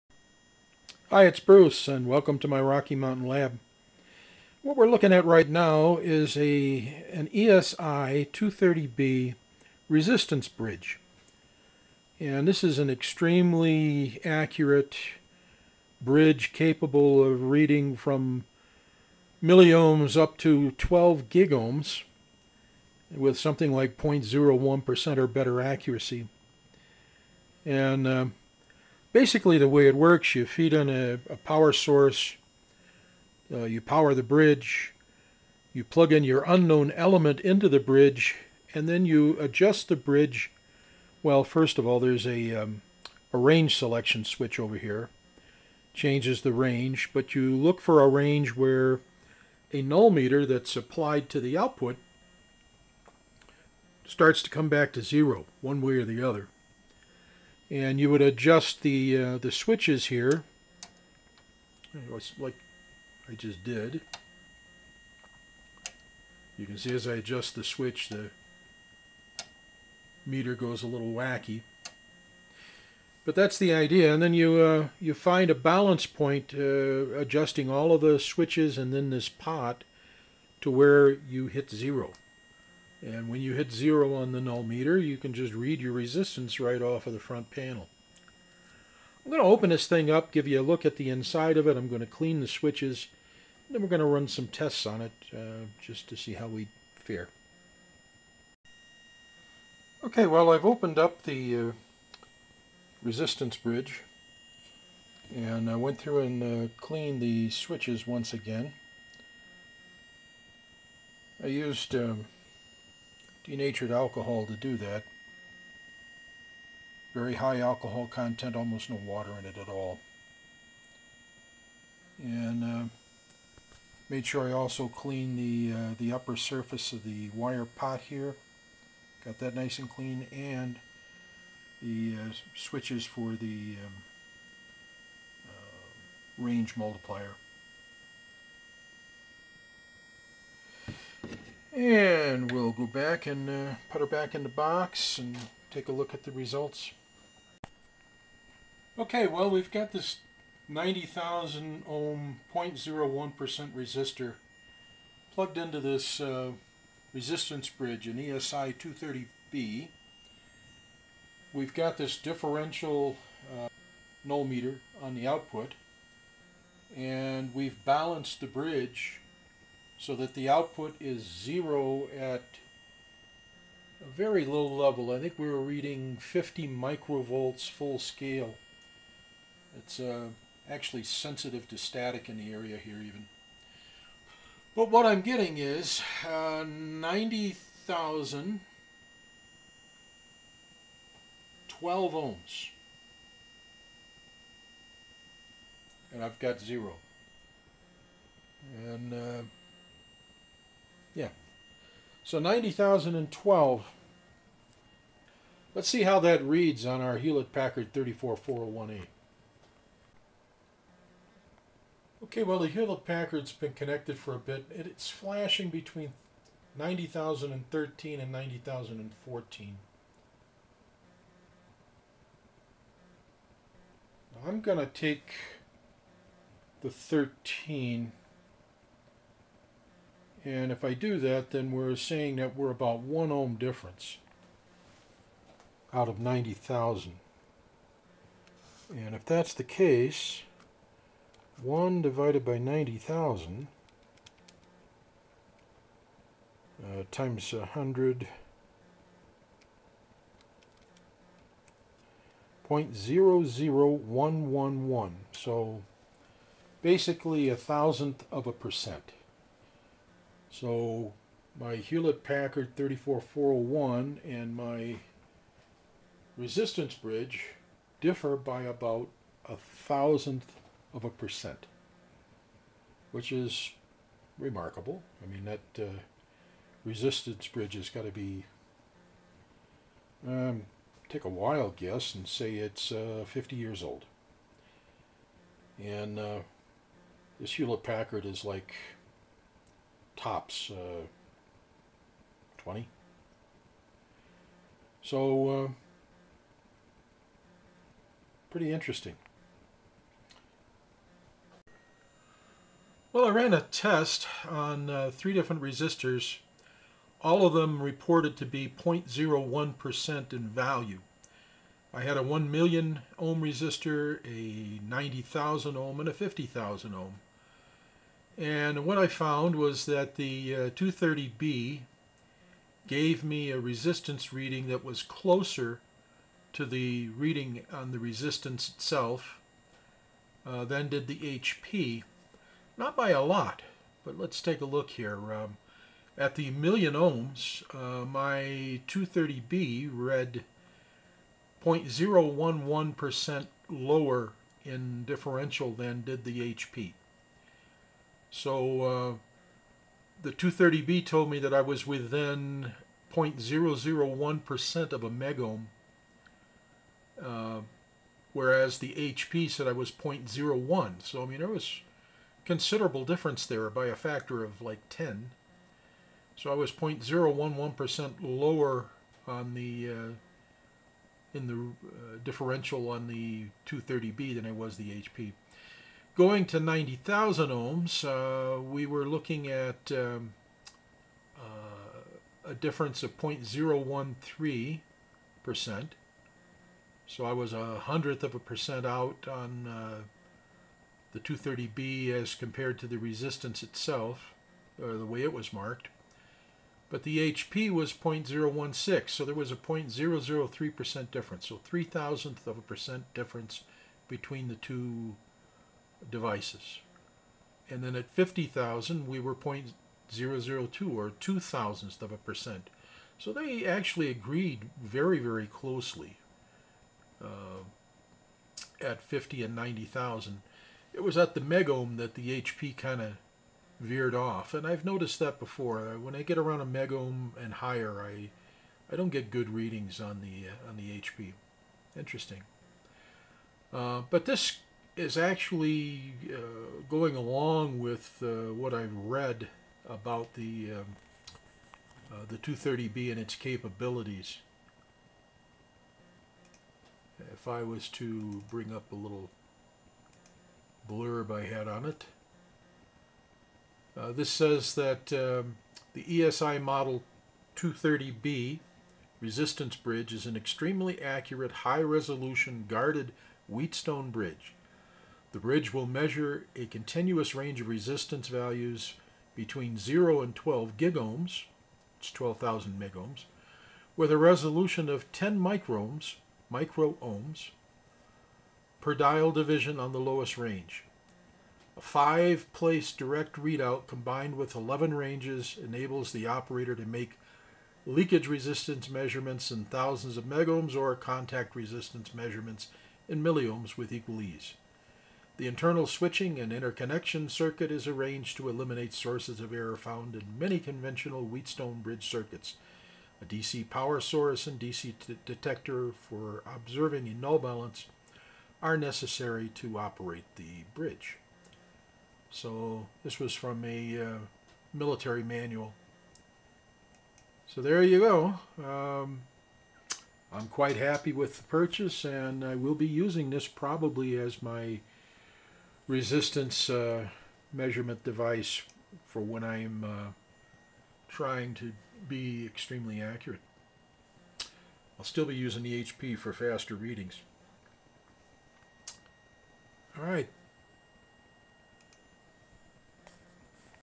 ESI 230B Resistance Bridge
I picked up one of these bridges a few months ago in hopes of getting it to work and study it.  Back in my early hobby years of the early 1960's, a unit like this would have been the means by which resistor batch (or even single unit) values were determined.

It is a unit that has the potential to determine resistances from micro-ohms to 12,000 megohms in value, to better than .01%.

In the video, I introduce the unit, open it up, show the interior, discuss the cleaning of the components and then I test the unit.  Using 3, .01% tolerance resistances, I measure them using the 230B and also my HP34401A and then I compare the readings.  The 230B comes out a champ!